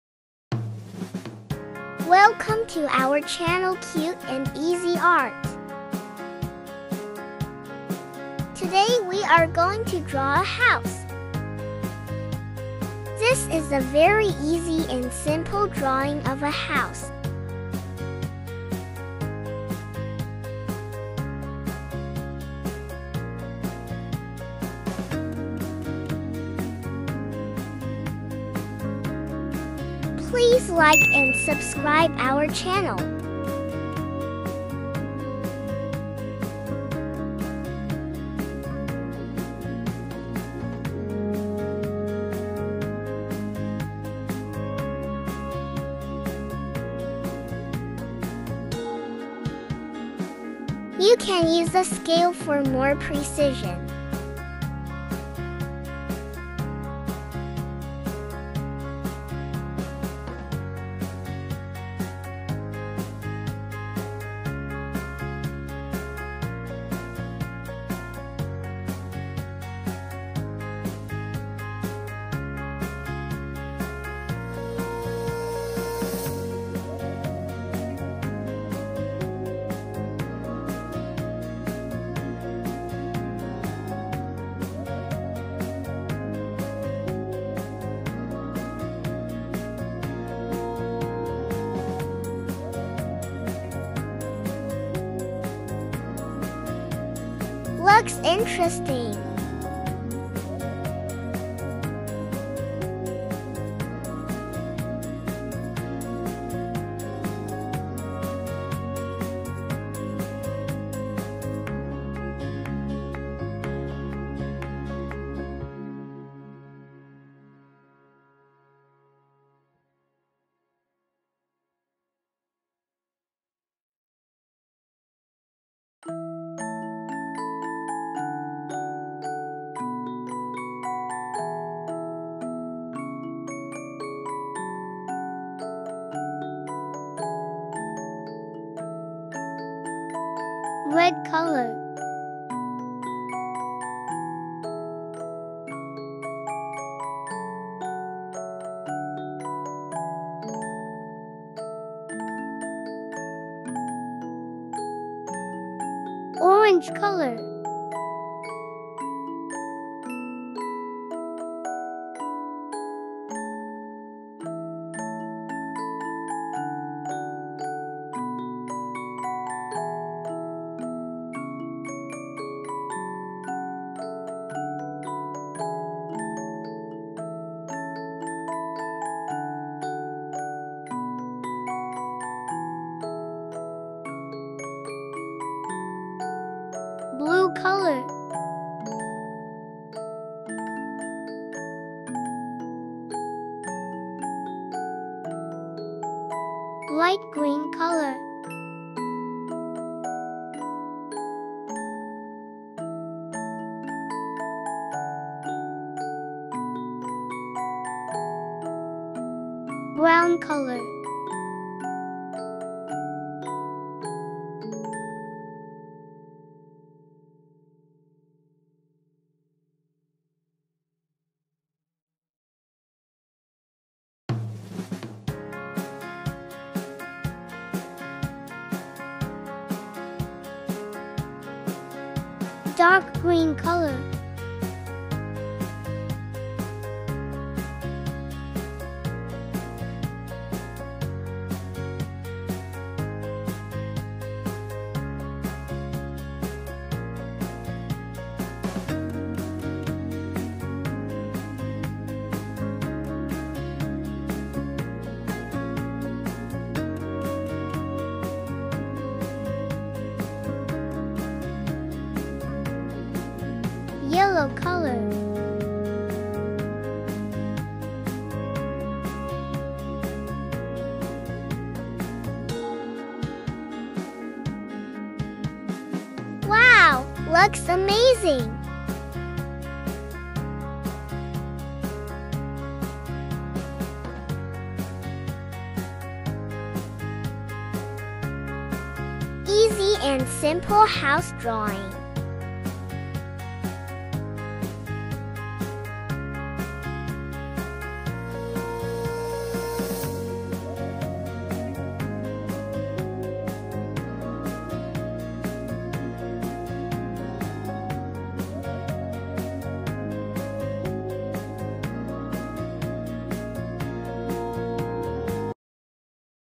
Very easy house drawing and painting for kids and beginners
Hi friends! Today we will learn how to draw and color a very easy and simple house. This is going to be a very simple and easy house drawing you’ll love to  draw and color! I love to draw and color a house and you? ❤️